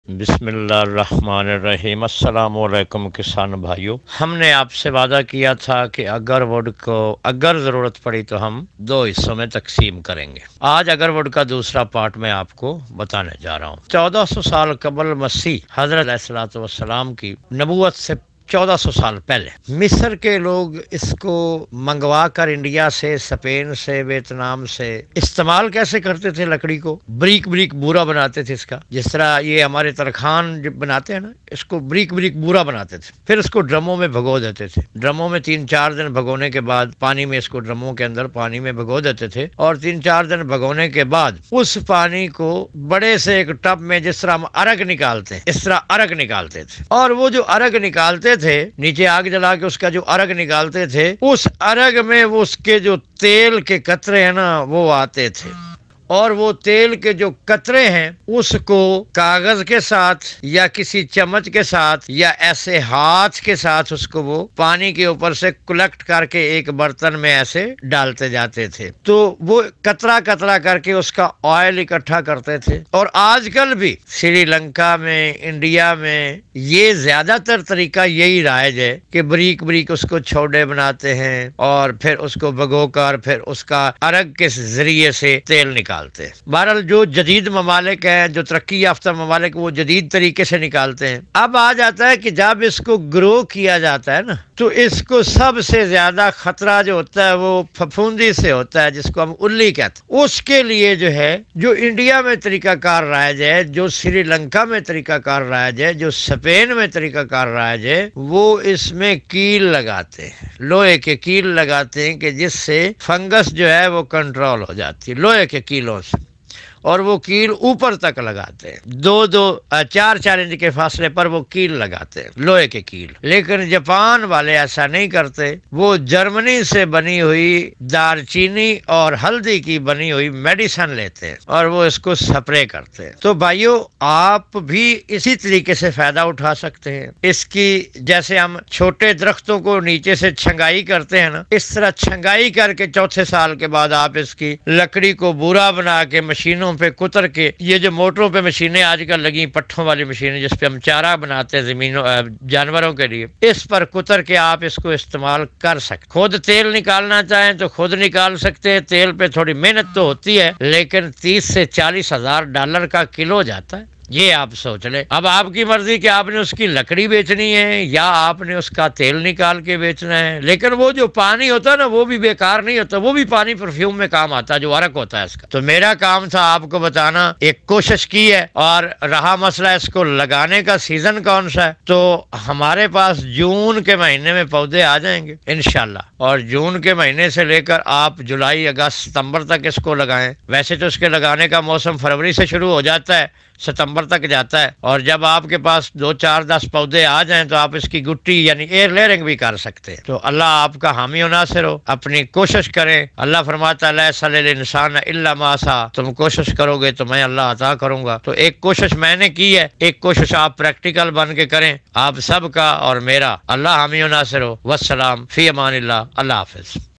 How to extract oil from Agarwood | PART-II Agarwood | Agarwood Ka Oil Nikalny Ka Tarika
This video is about Agarwood oil extraction. Agarwood PART-II. Agarwood Ka Oil Nikalny Ka Tarika.  How to extract oil from Agarwood.